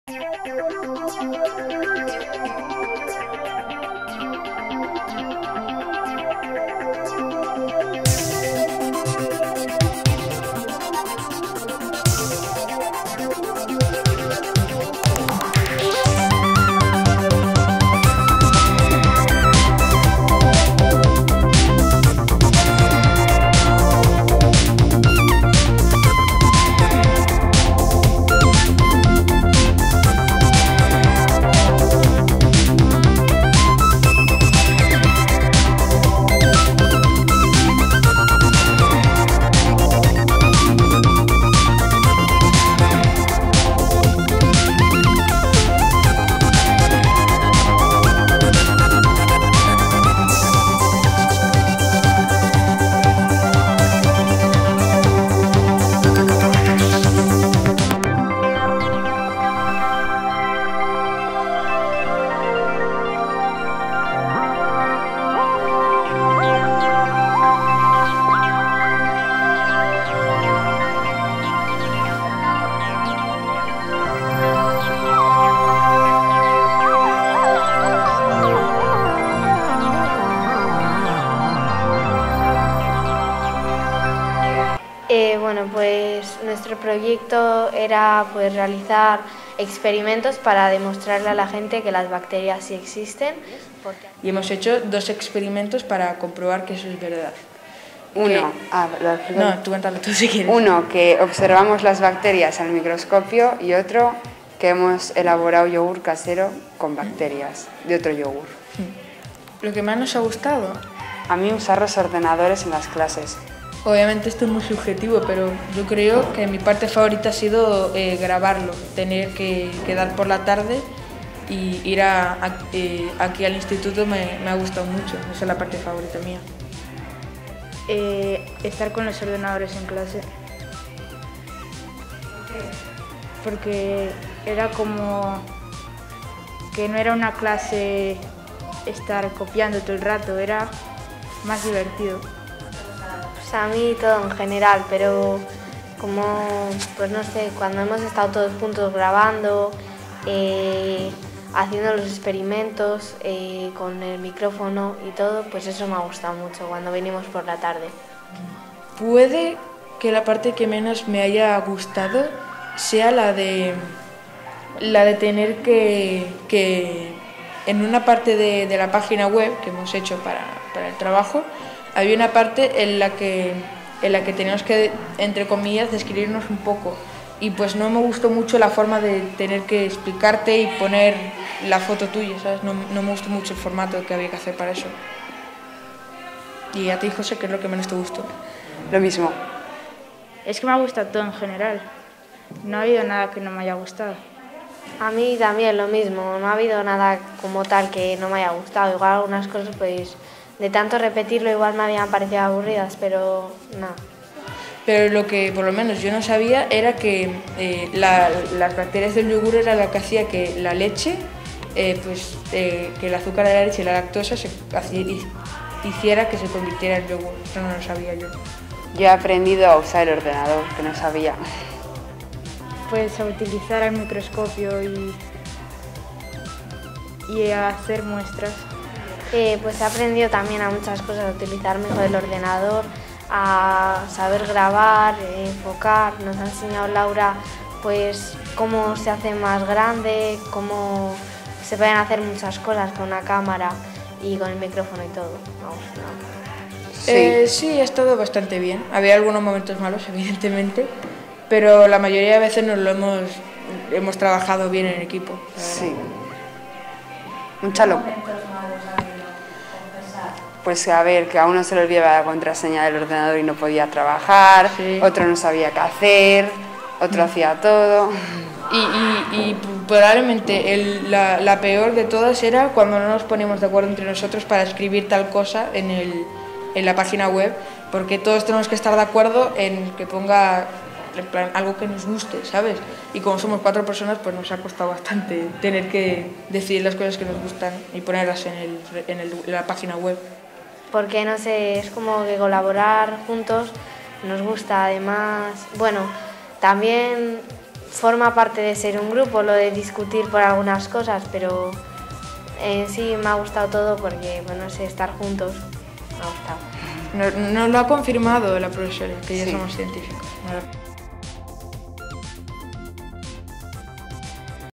¿Cómo se hizo?: ¿Existen las bacterias?
Resumen de proyecto "¿Existen las bacterias?" realizado por alumnos de 1º de ESO del IES Marismas de Santoña.